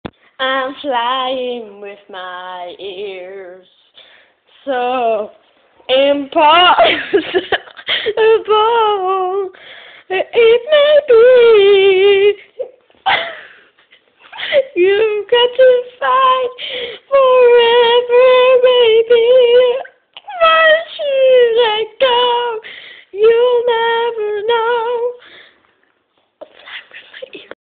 im flying with my ears .
my dog sasha singing flying with my ears x.